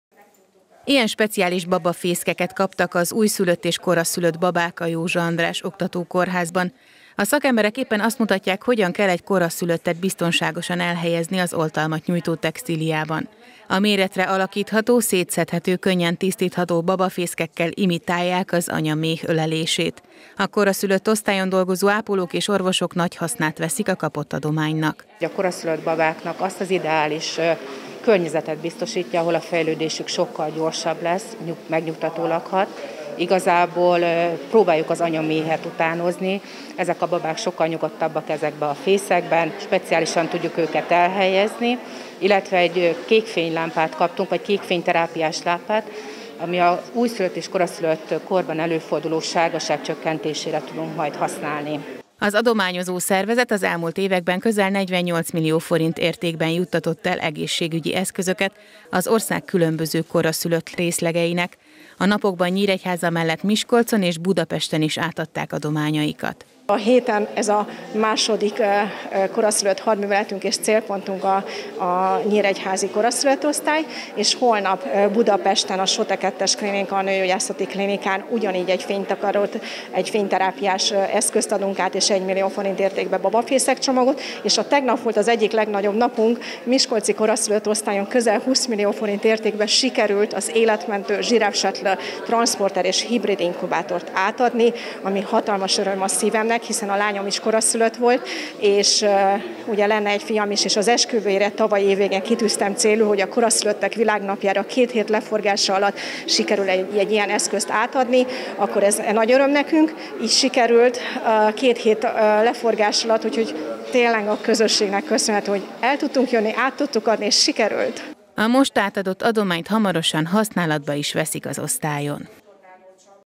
Adomány a kórháznak
Egy kékfényterápiás eszközt és babafészkeket kapott a Jósa András Oktatókórház koraszülött részlege. A közel kétmillió forint értékű adományt az Alapítvány az Alapítványokért Plüss Kommandója hozta el a nyíregyházi intézménybe. A kórházban évek óta használnak ilyen speciális eszközöket, amelyek az ápolási és gyógyítási munkákat segíti.